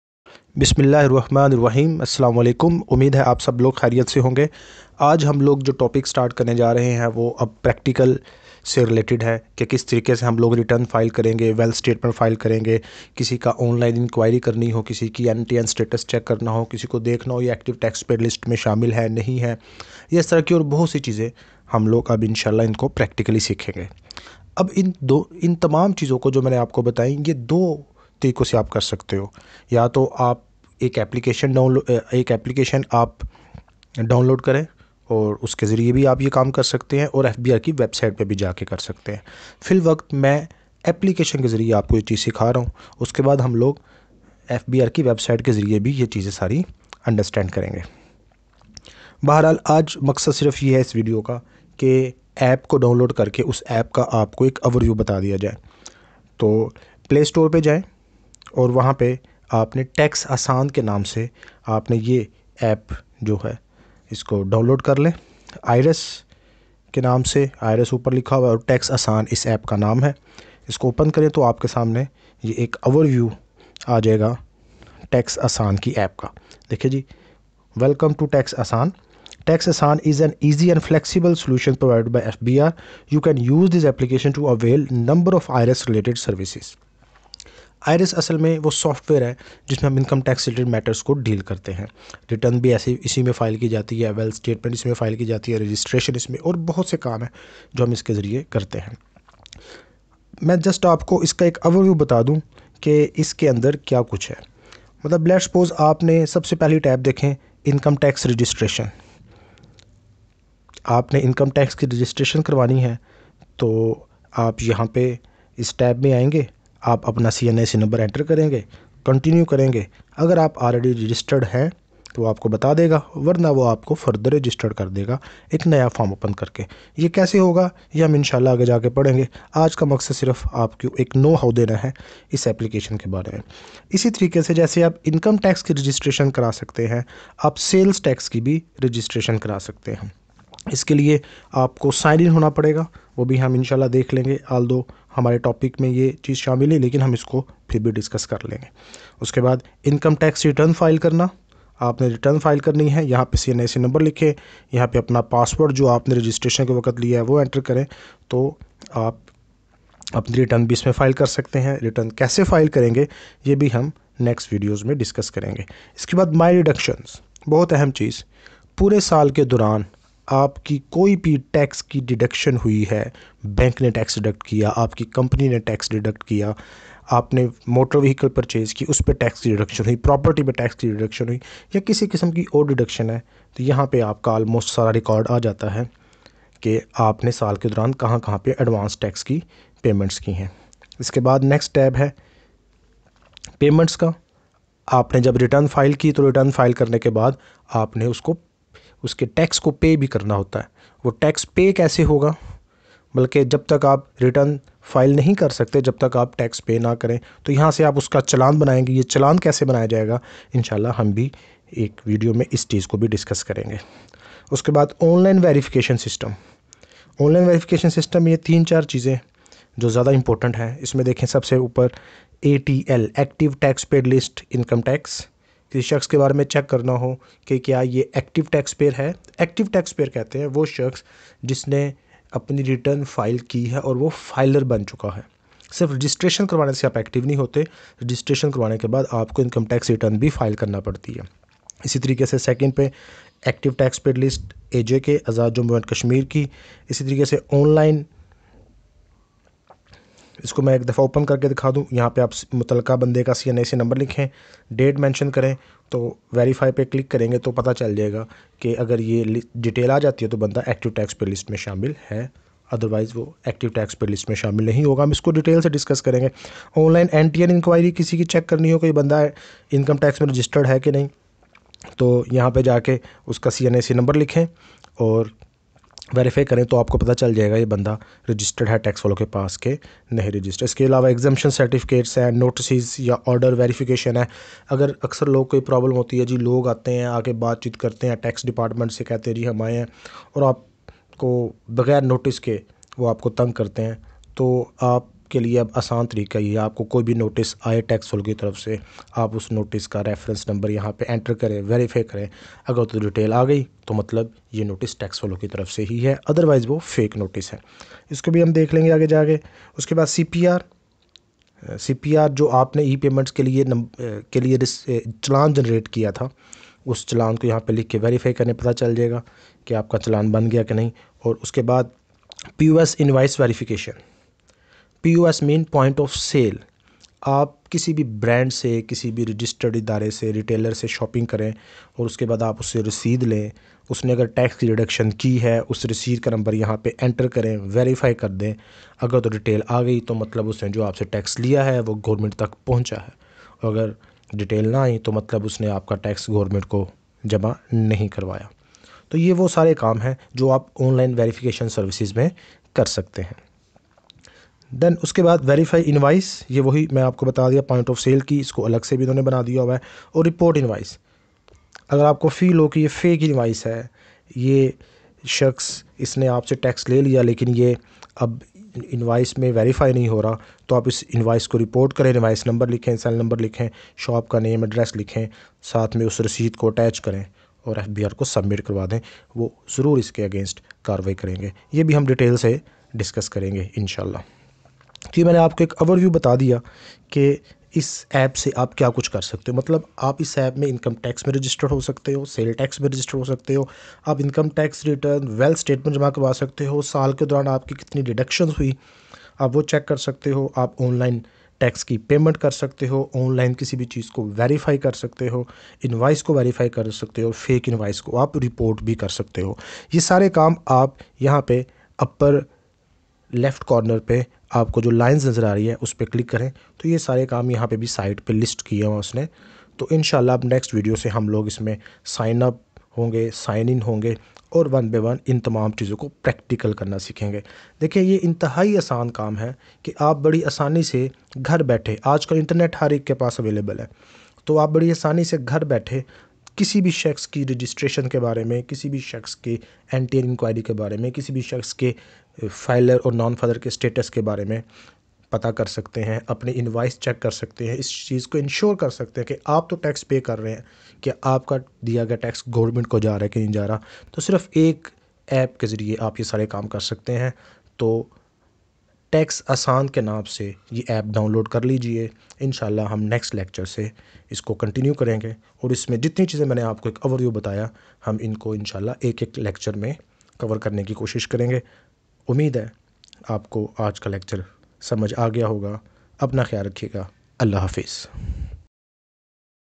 Overview of Tax Asaan App | Saad Anwar Mughal
This lecture is about the overview of Tax Asaan App and we will understand the various options available in the Tax Assan App.